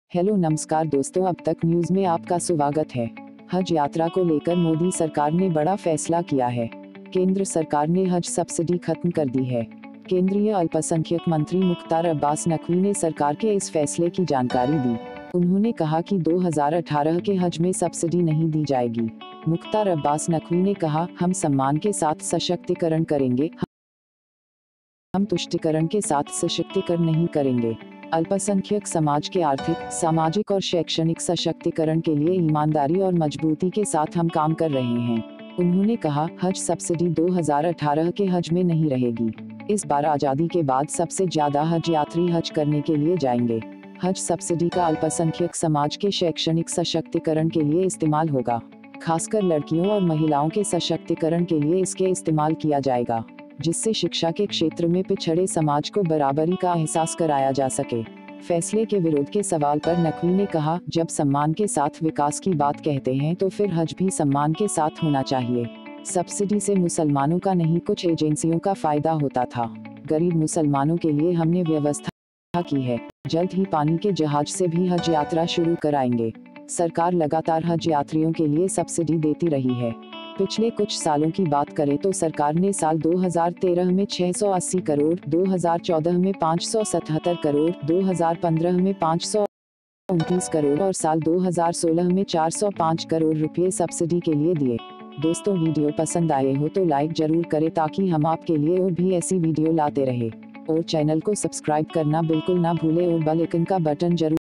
मोदी सरकार का बड़ा फैसला, हज यात्रा के लिए अब नहीं मिलेगी सब्सिडी
हज यात्रा को लेकर मोदी सरकार ने बड़ा फैसला किया है। केंद्र सरकार ने हज सब्सिडी खत्म कर दी है। केंद्रीय अल्पसंख्यक मंत्री मुख्तार अब्बास नकवी ने सरकार के इस फैसले की जानकारी दी। उन्होंने कहा कि 2018 के हज में सब्सिडी नहीं दी जाएगी।

मुख्तार अब्बास नकवी ने कहा, ''हम सम्मान के साथ सशक्तिकरण करेंगे, हम तुष्टिकरण के साथ सशक्तिकर नहीं करेंगे। अल्पसंख्यक समाज के आर्थिक, सामाजिक और शैक्षणिक सशक्तिकरण के लिए ईमानदारी और मजबूती के साथ हम काम कर रहे हैं।''

उन्होंने कहा, ''हज सब्सिडी 2018 के हज में नहीं रहेगी। इस बार आजादी के बाद सबसे ज्यादा हज यात्री हज करने के लिए जाएंगे। हज सब्सिडी का अल्पसंख्यक समाज के शैक्षणिक सशक्तिकरण के लिए इस्तेमाल होगा। खासकर लड़कियों और महिलाओं के सशक्तिकरण के लिए इसके इस्तेमाल किया जाएगा। जिससे शिक्षा के क्षेत्र में पिछड़े समाज को बराबरी का अहसास कराया जा सके।''

फैसले के विरोध के सवाल पर नकवी ने कहा, ''जब सम्मान के साथ विकास की बात कहते हैं तो फिर हज भी सम्मान के साथ होना चाहिए। सब्सिडी से मुसलमानों का नहीं कुछ एजेंसियों का फायदा होता था। गरीब मुसलमानों के लिए हमने व्यवस्था की है, जल्द ही पानी के जहाज से भी हज यात्रा शुरू कराएंगे।''

सरकार लगातार हज यात्रियों के लिए सब्सिडी देती रही है। पिछले कुछ सालों की बात करें तो सरकार ने साल 2013 में 680 करोड़, 2014 में 577 करोड़, 2015 में 529 करोड़ और साल 2016 में 405 करोड़ रुपये सब्सिडी के लिए दिए